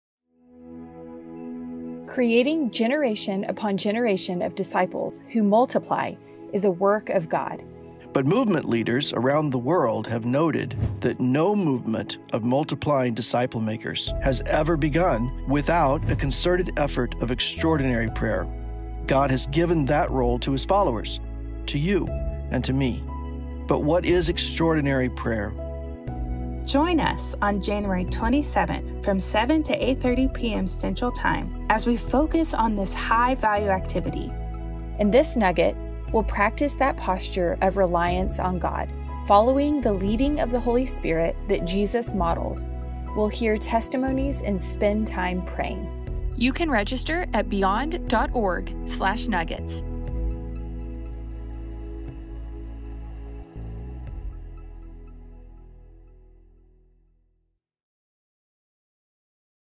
Nugget Promo - Extraordinary Prayer: Spiritual Posture of Disciple Makers
Creating generation upon generation of disciples who multiply is a work of God. But movement leaders around the world have noted that no movement of multiplying disciple-makers has ever begun without a concerted effort of extraordinary prayer. God has given that role to His followers - to you and me!

But what is extraordinary prayer? Join us on January 27th from 7-8:30 pm (CST) as we focus on this high value activity. In this Nugget, we’ll learn about practicing that posture of reliance on God, following the lead of the Holy Spirit that Jesus modeled. We'll hear testimonies and spend time praying.